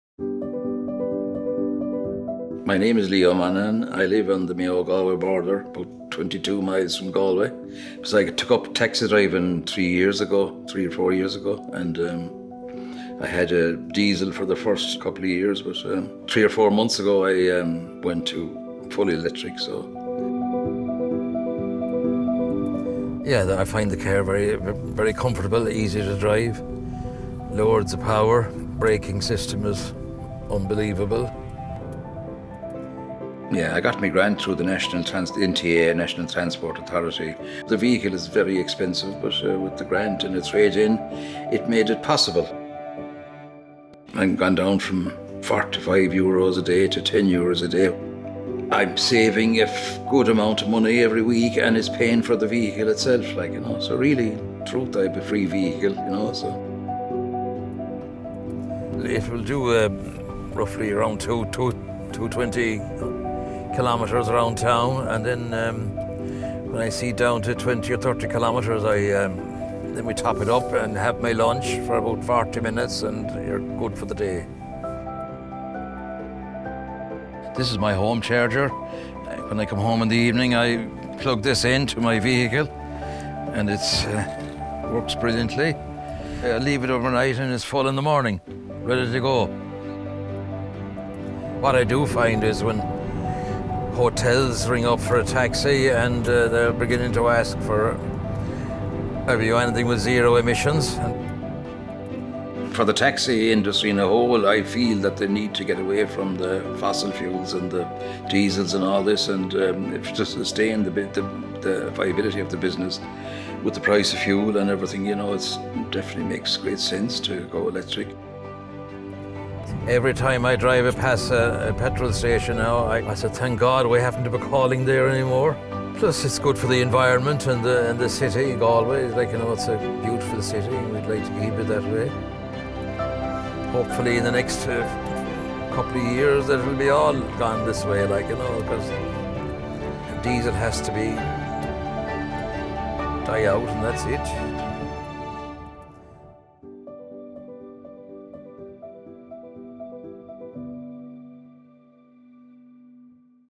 Climate Action Plan – Transport/Electric Vehicles
Leo Monaghan is a taxi driver, living on the Mayo Galway border. Here he talks about the benefits of moving to an electric vehicle.